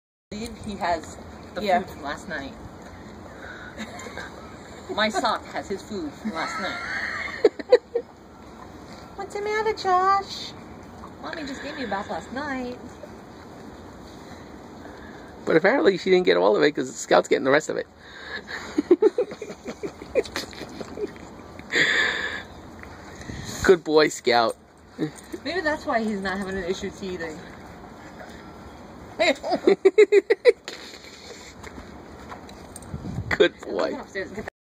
Malamute attacks baby
A 3 year old malamute attacks a baby without reason